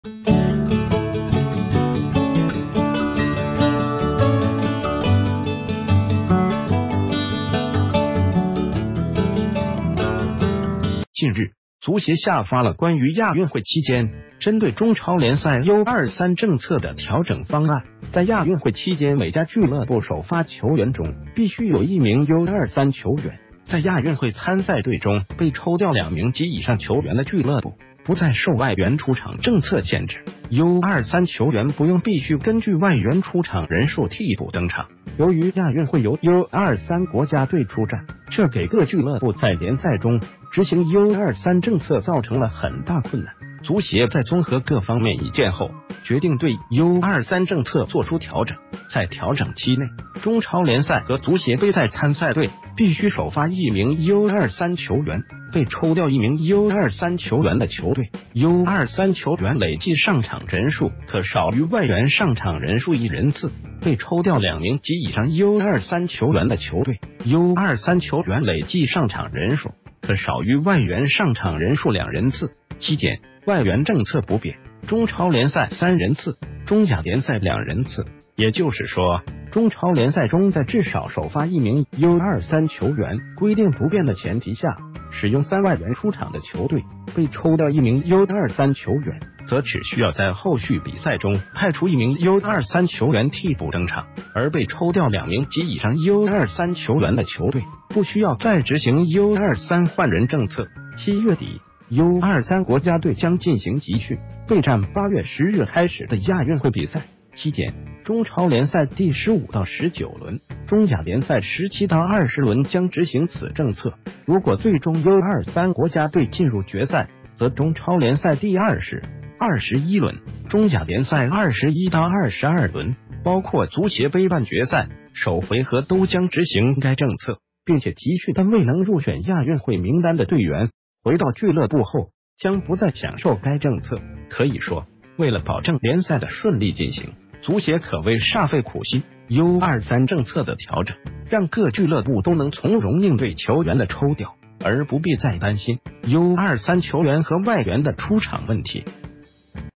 足協調整亞運會U23政策：首發1人 抽調2人以上俱樂部不受換人限制
近日，足協下發了關于亞運會期間針對中超聯賽U23政策的調整方案，在亞運會期間每家俱樂部首發球員中必須有一名U23球員，在亞運會參賽隊中被抽調2名及以上球員的俱樂部，不再受外援出場政策限制，U23球員不用必須根據外援出場人數替補登場。
    由于亞運會由U23國家隊出戰，這給各俱樂部在聯賽中執行U23政策造成了很大困難，足協在綜合各方面意見后，絕定對U23政策做出調整。在調整期內，中超聯賽和足協杯賽參賽隊必須首發一名U23球員，被抽調1名U23球員的球隊，U23球員累計上場人數可少于外援上場人數1人次，被抽調2名及以上U23球員的球隊，U23球員累計上場人數，可少于外援上場人數2人次。期間，外援政策不變，中超聯賽三人次，中甲聯賽兩人次。
    也就是說，中超聯賽中在至少首發1名U23球員規定不變的前提下使用三外援出場的球隊被抽調1名U23球員，則只需要在后續比賽中派出1名U23球員替補登場，而被抽調2名及以上U23球員的球隊，不需要再執行U23換人政策。
    7月底，U23國家隊將進行集訓，備戰8月10日開始的亞運會比賽，期間，中超聯賽第15-19輪，中甲聯賽17-20輪將執行此政策，如果最終U23國家隊進入決賽，則中超聯賽第20-21輪，中甲聯賽21-22輪，包括足協杯半決賽首回合都將執行該政策。并且集訓但未能入選亞運會名單的隊員，回到俱樂部后將不再享受該政策。
    可以說，為了保證聯賽的順利進行，足協可謂煞費苦心，U23政策的調整，讓各俱樂部都能從容應對球員的抽調，而不必再擔心，U23球員和外援的出場問題。